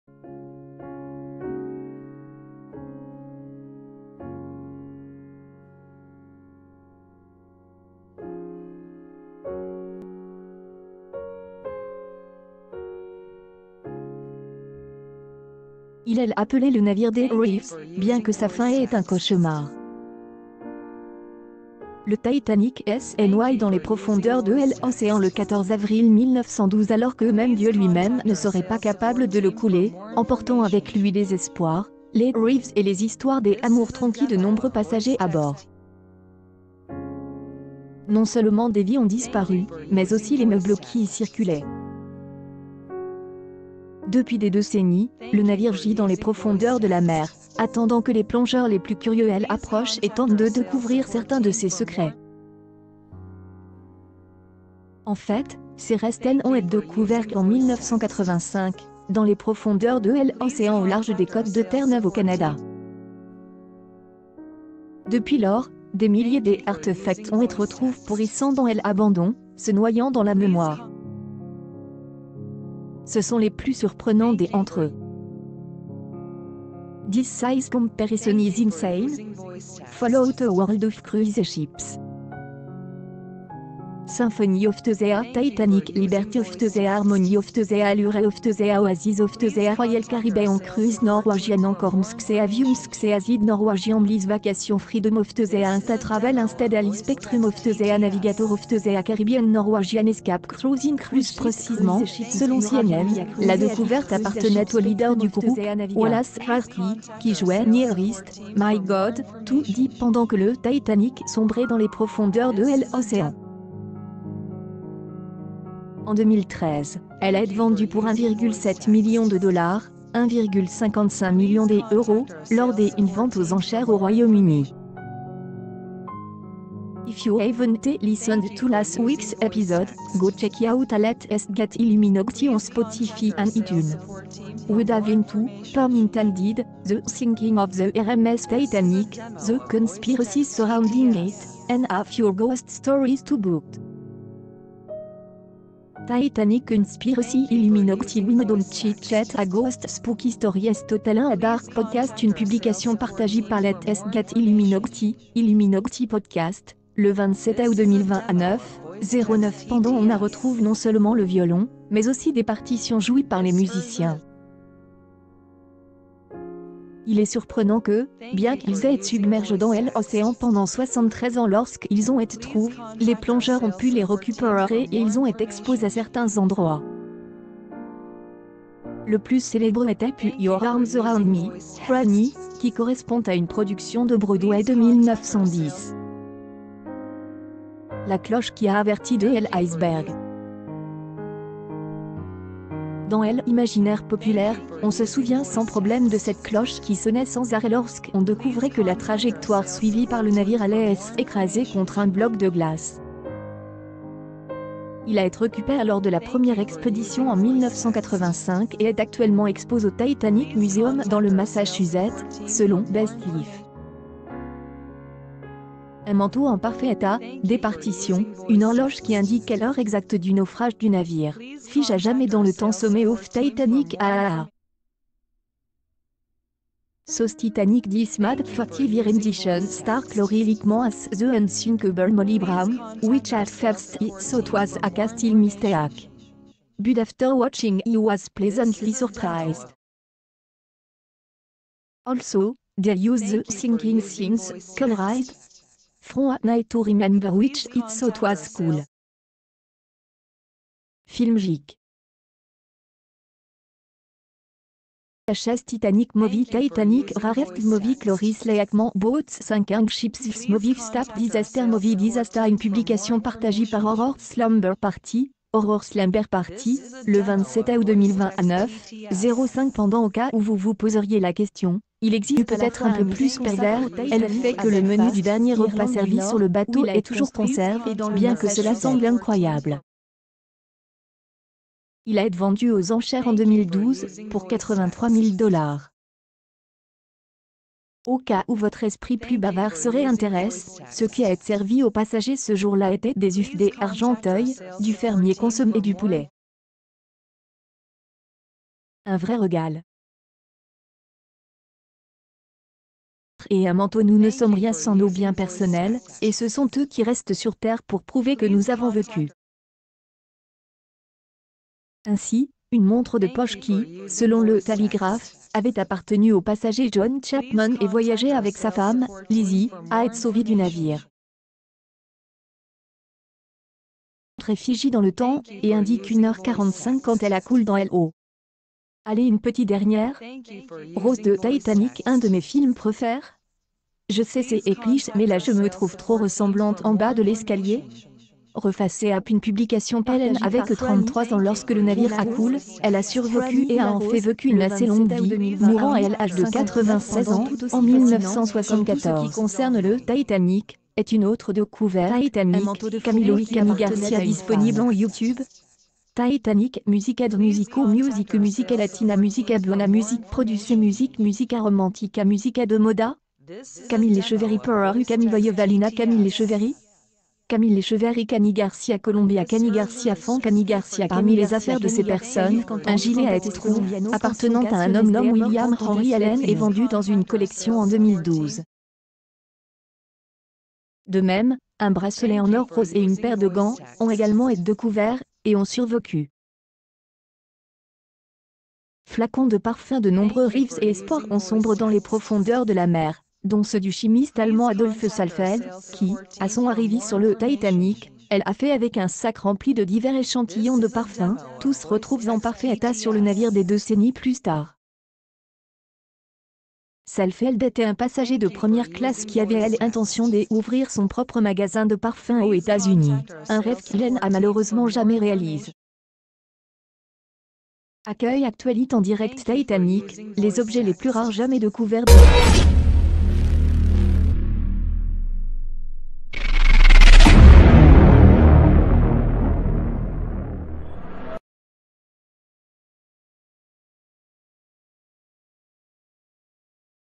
Titanic : Les objets les plus rares jamais découverts dévoilés.
,#les 10,#les 10,#épouses les plus inhabituelles,#les personnes les plus étranges,#les objets de mon enfance
Titanic : Les objets les plus rares jamais découverts dévoilés. Ils l’appelaient le navire des rêves, bien que sa fin ait été un cauchemar. Le Titanic s’est noyé dans les profondeurs de l’océan le